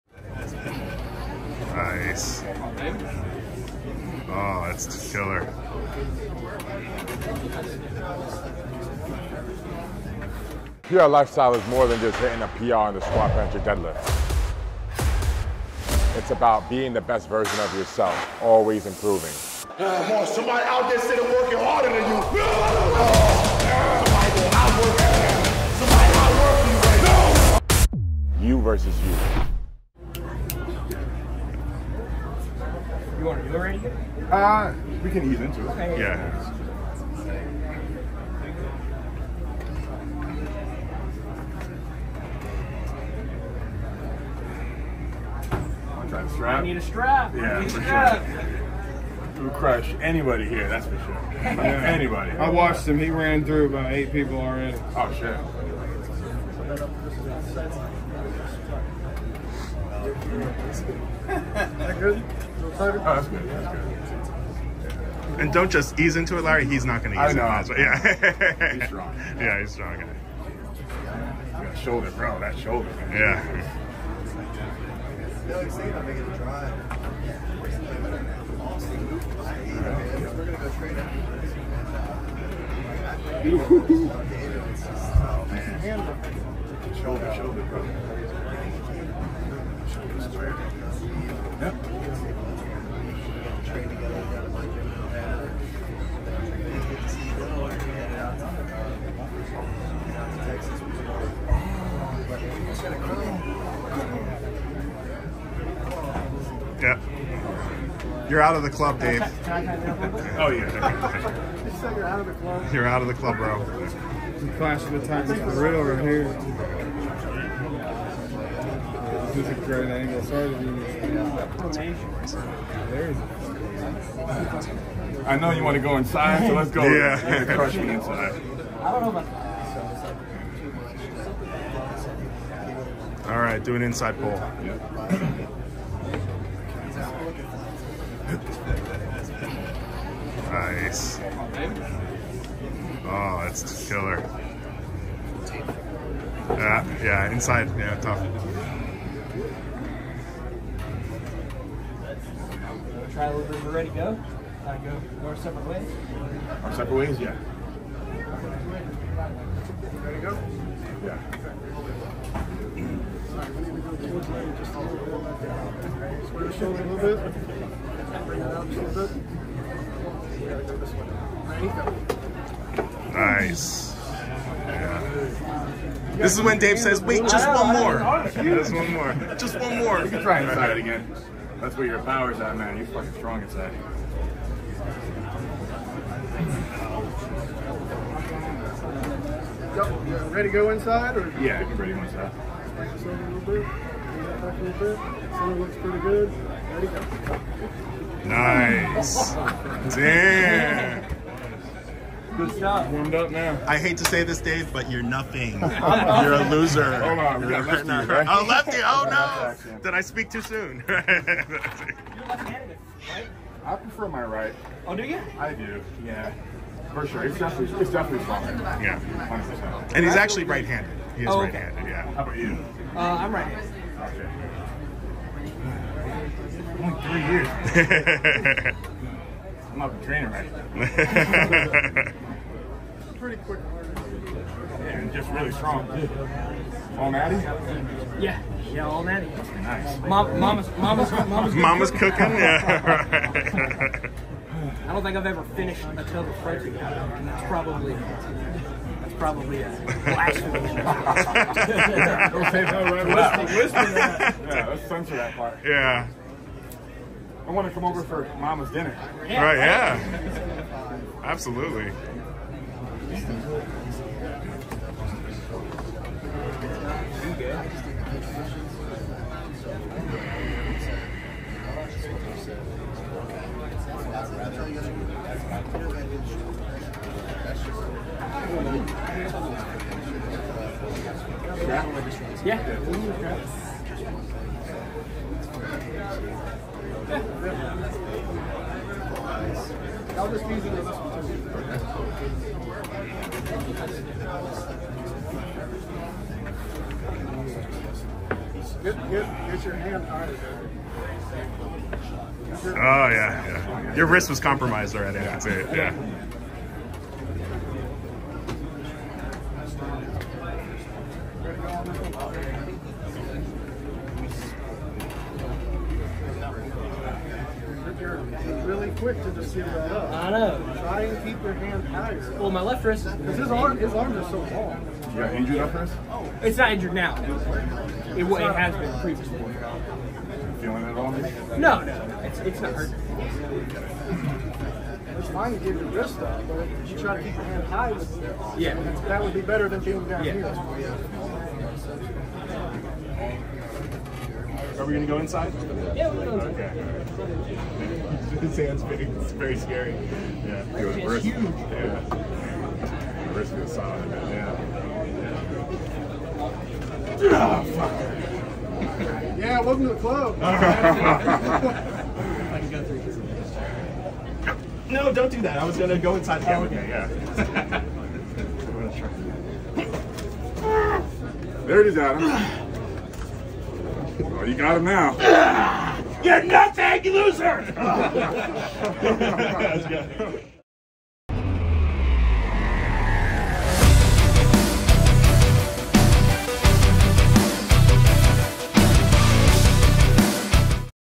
Arm Wrestling with The Strongest Teen Arm Wrestler in The US!
Code: WHEELS